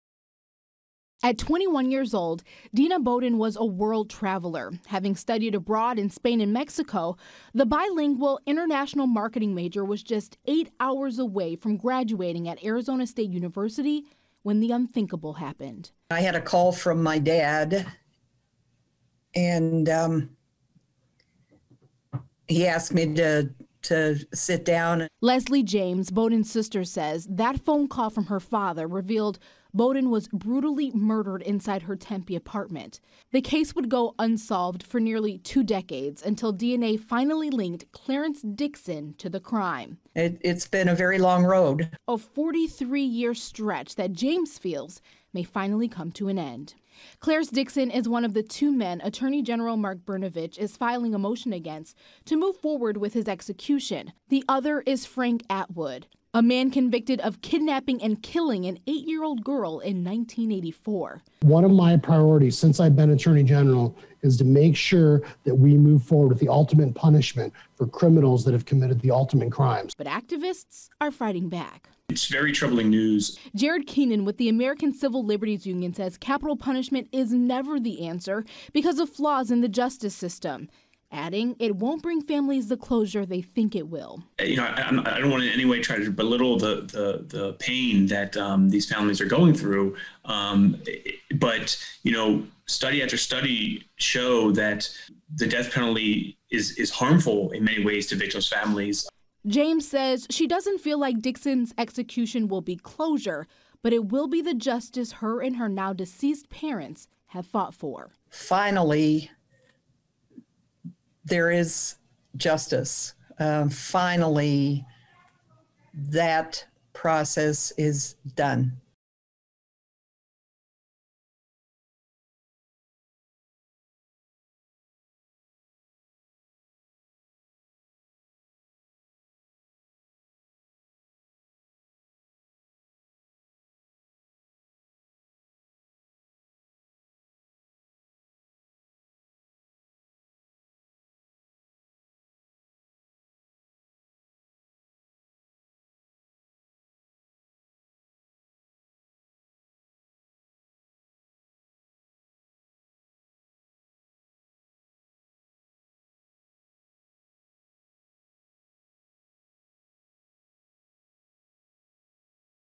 Arizona plans to seek warrants for 1st executions since 2014
Arizona Attorney General Mark Brnovich filed motions on Tuesday to move forward with the executions of two men currently on death row.